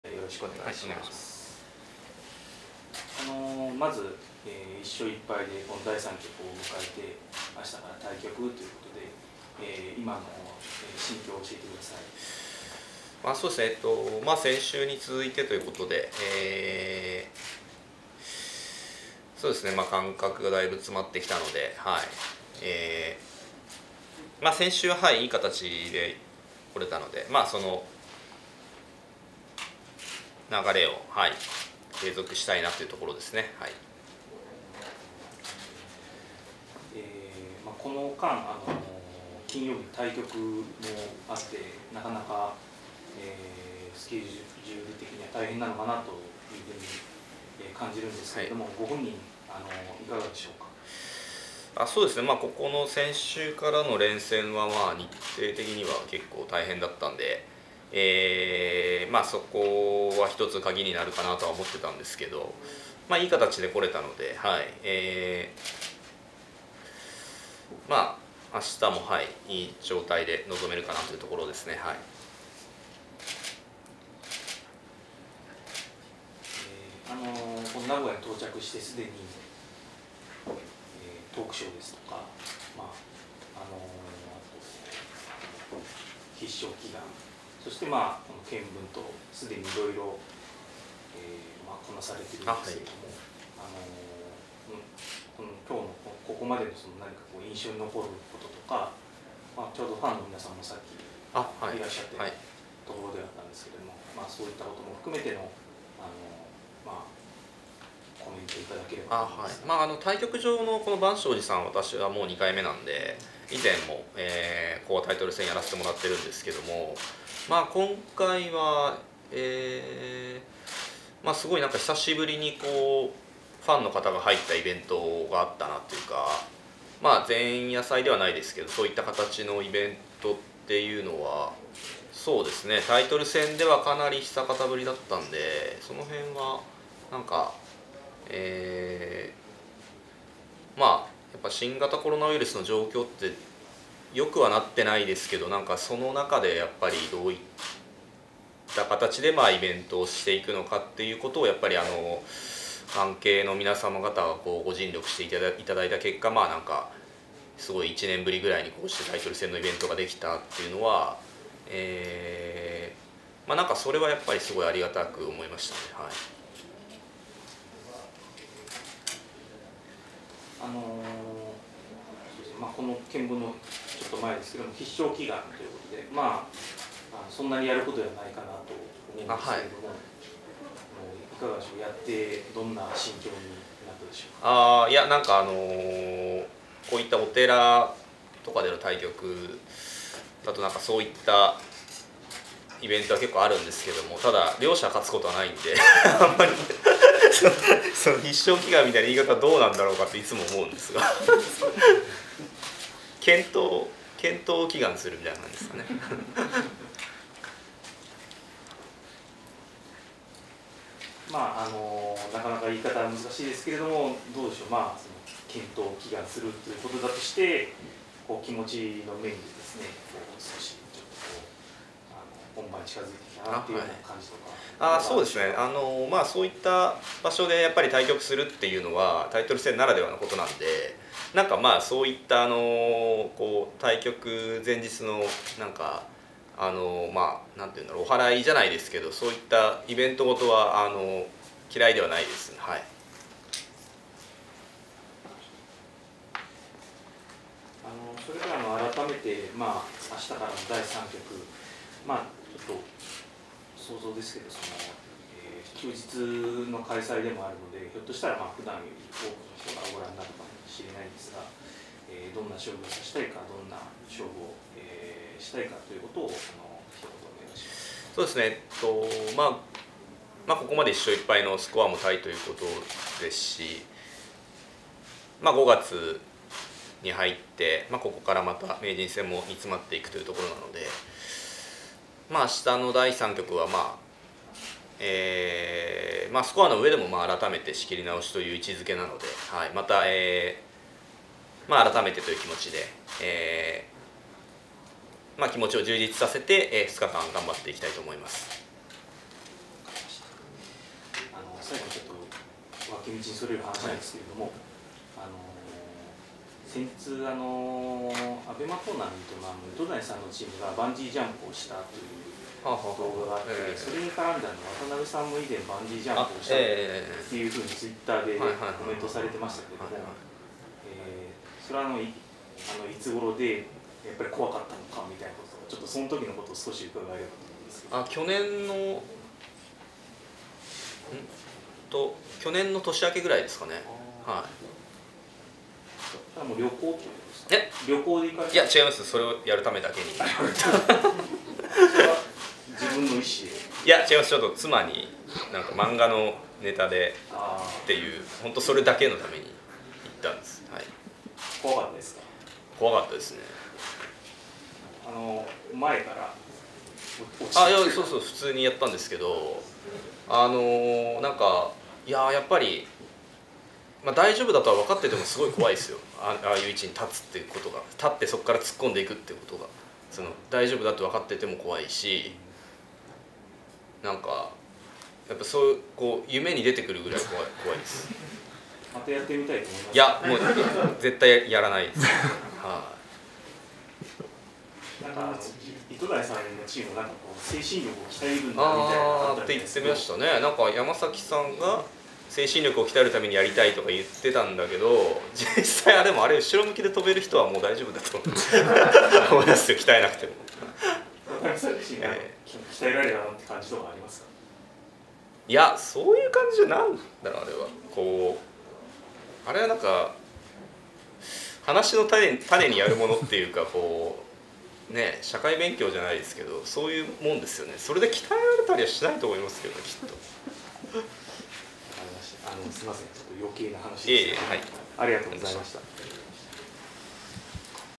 渡辺名人、バンジージャンプ「もうやりたくない…」対局前夜にリラックス【第79期将棋名人戦第3局】＝高津祐典撮影
渡辺明名人に斎藤慎太郎八段が挑戦する第79期将棋名人戦第３局。祈禱や検分を終えて対局前日のインタビューに応じた渡辺名人は、リラックスした表情に。バンジージャンプをしたことを振り返り「もうやりたくない」と笑顔を見せた。